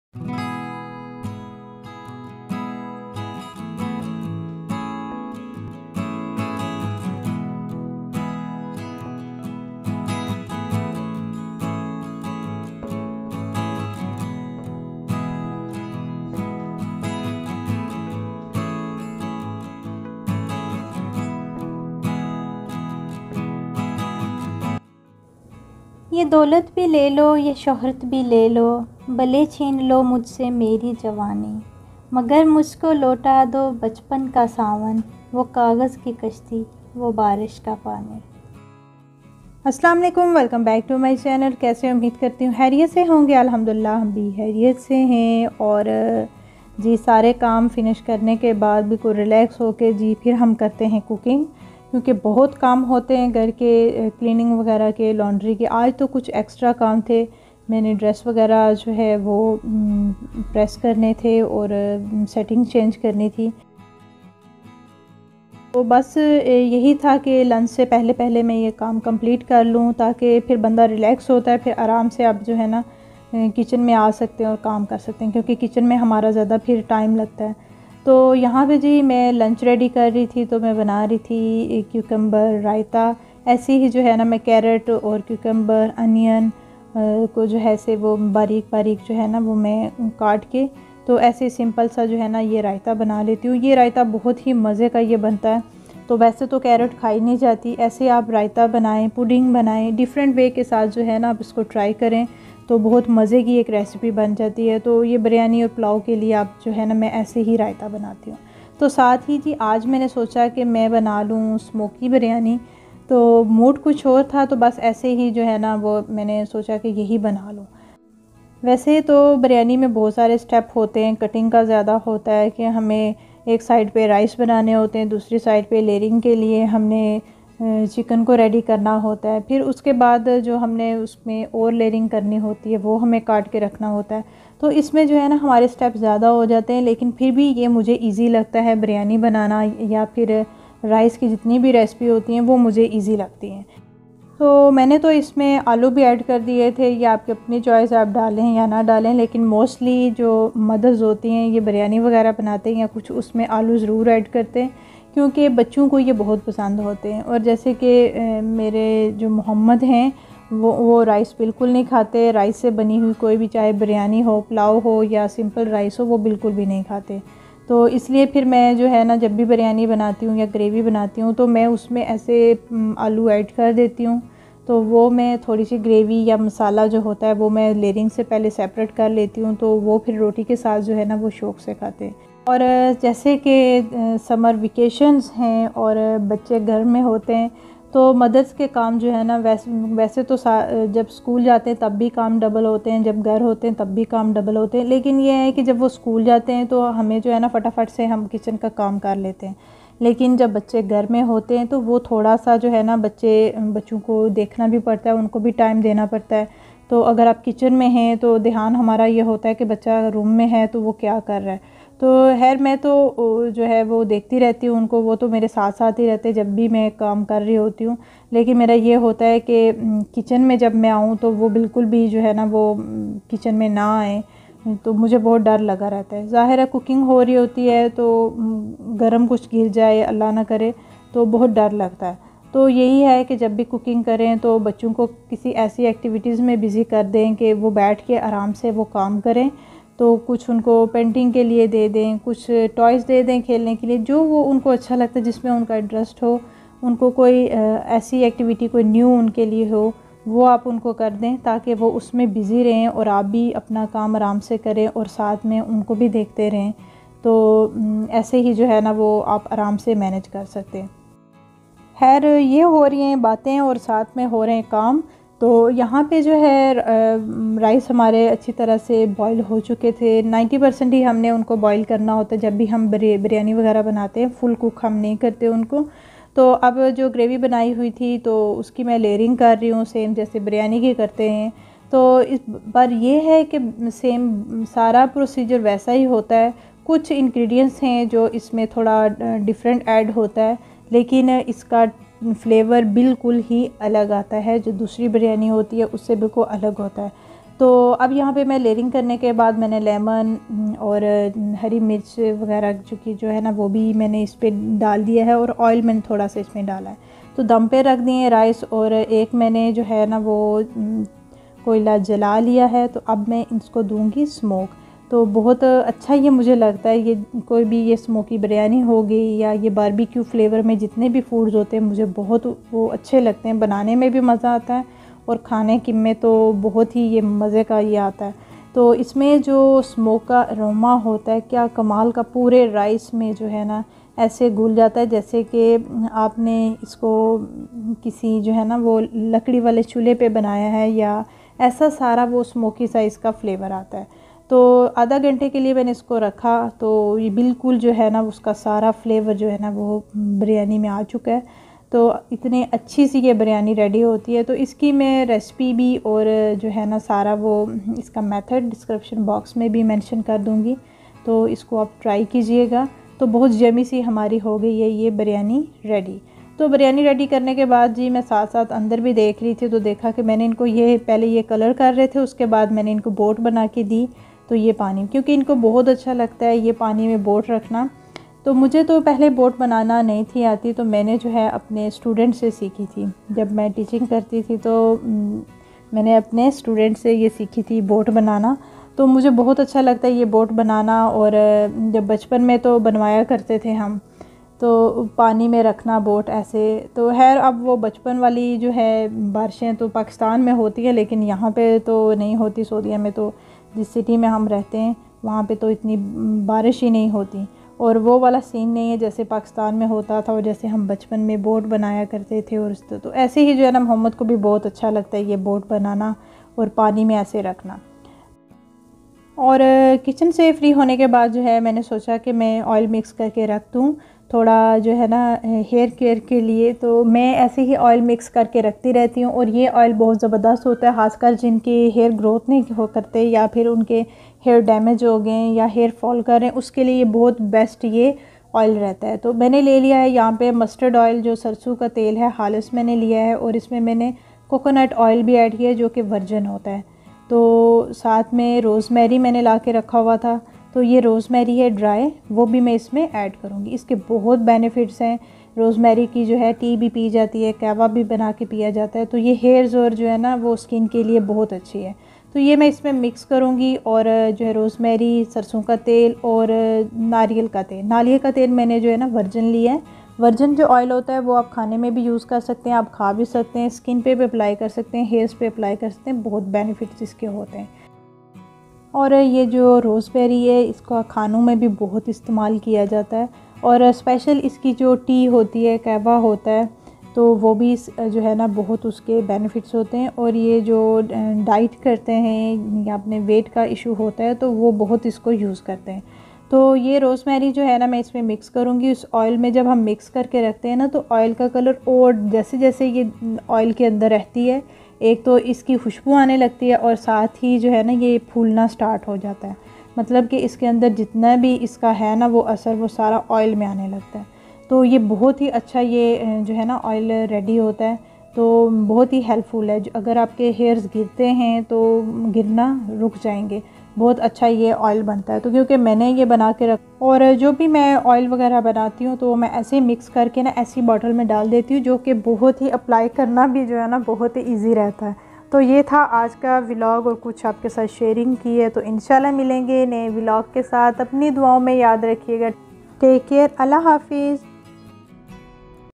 Smokey Biryani ! Quick Cucumber Raita !! Natural Hair care Tips!! Aj phr sy bachpan ki yad ai
Music Credits 👇
Youtube Audio music library

Other video link 👇
Lunch routine